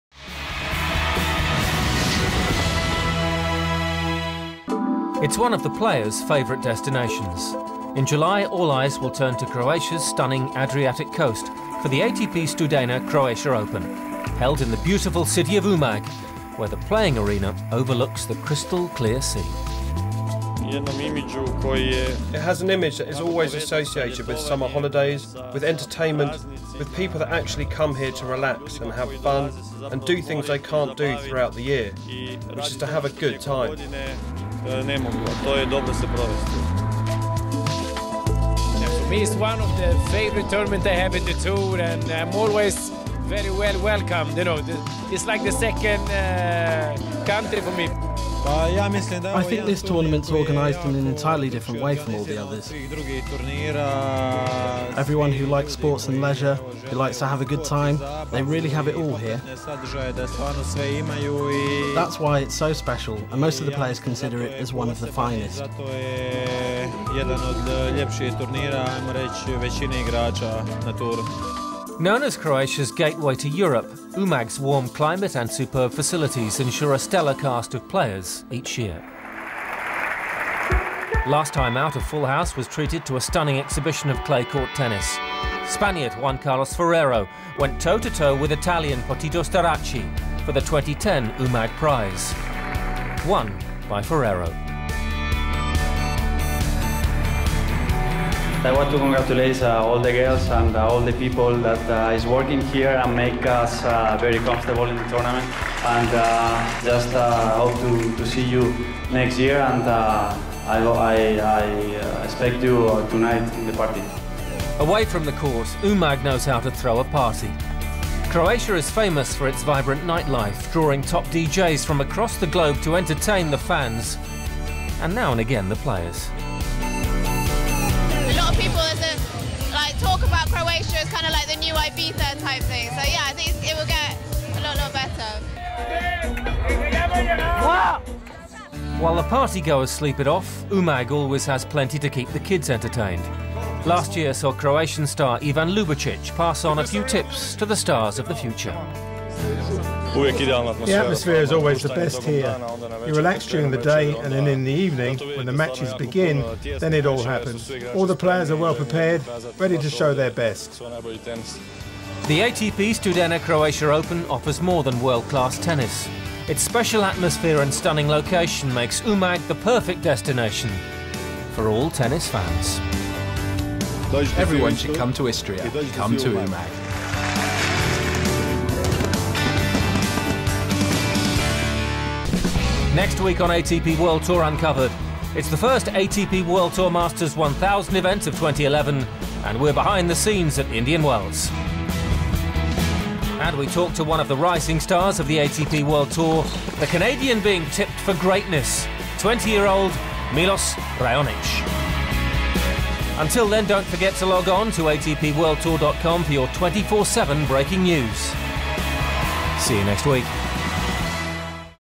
ATP world tour uncovered_21. Studena Croatia Open Umag
Take a look at the «ATP world tour uncovered» Youtube video about Umag. In this film you can see why it is our tournament one of the most favorite to all of the players. ATP Studena Croatia Open will confirm the status of one of the most important social and sports events in Croatia. During the tournament Umag becomes an unavoidable summer centre of tennis, entertainment and social activities. ATP Studena Croatia Open Umag has an image that is associated with summer holidays, with entertainment and with people that actually come here to relax and have fun while doing things they can't do through the year: TO HAVE A GOOD TIME! -- you can check that out by clicking on this movie!